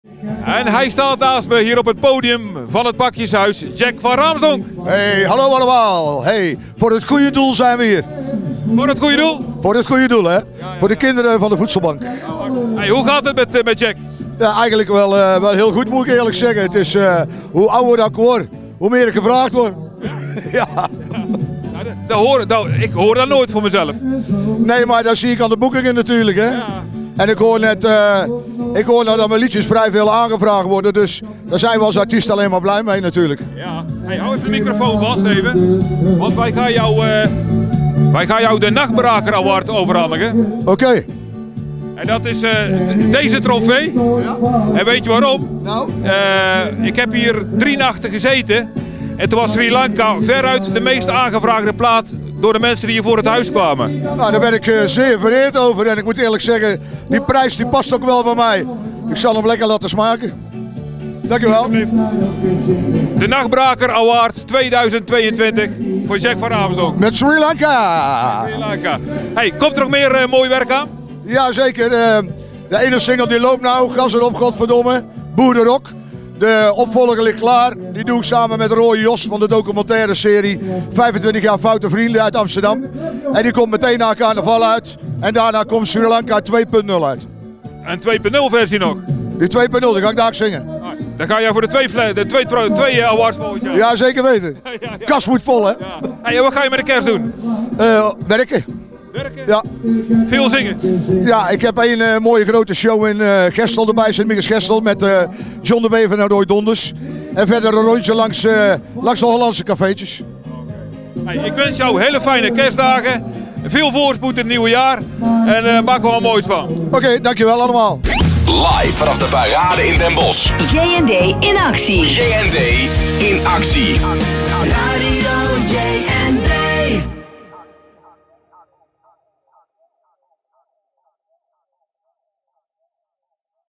JND In Actie | Jack Van Raamsdonk 18-12-2022
Jack van Raamsdonk was erbij op de Bossche middag tijdens JND In Actie